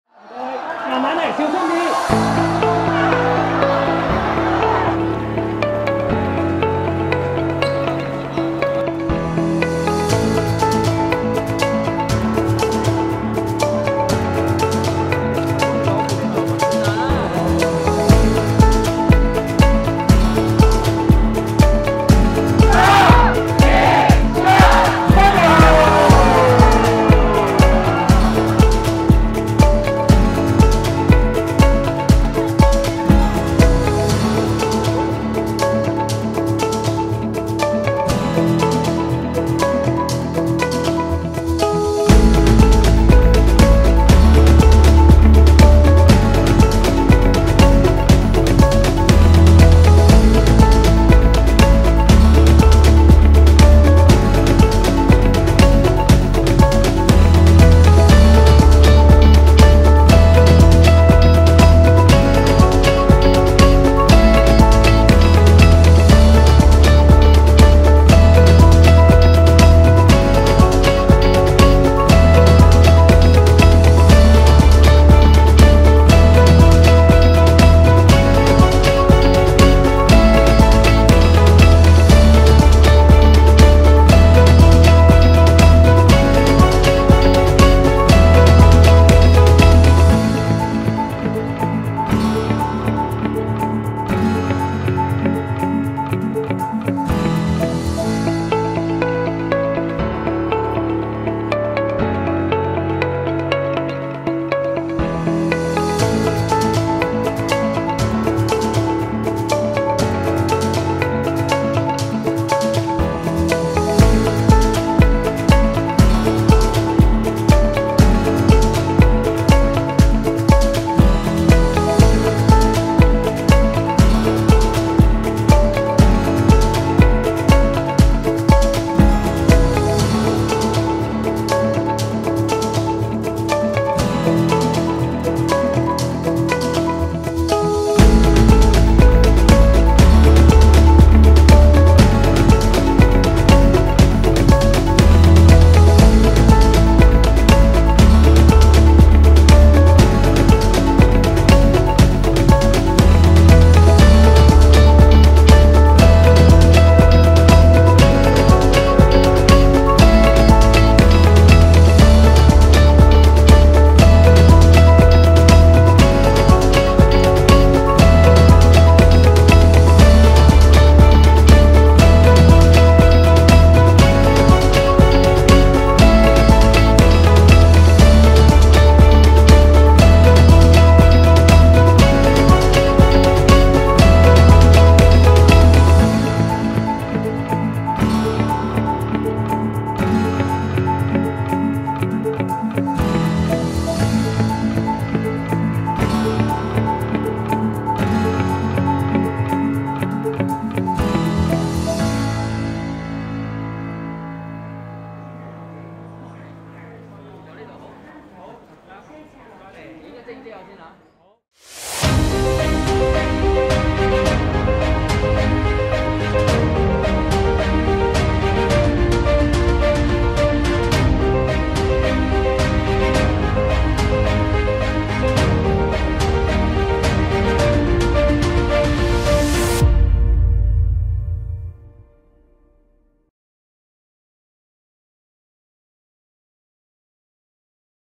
屯馬開通真的很興奮？港鐵屯馬綫全綫通車花絮
港鐵屯馬綫全綫於2021年6月27日通車，當日首班列車於上午5時50分在宋皇臺站開往屯門，吸引不少市民前來乘搭。同時，宋皇臺站、土瓜灣站及何文田站的屯馬綫月台亦於同日正式投入服務，紅磡站的屯馬綫月台則於一星期前的6月20日啟用。西鐵綫（前身為「九廣西鐵」）及屯馬綫一期（前身為「馬鞍山鐵路」、「馬鞍山綫」）正式合併為屯馬綫，兩綫的名稱亦隨之走進歷史。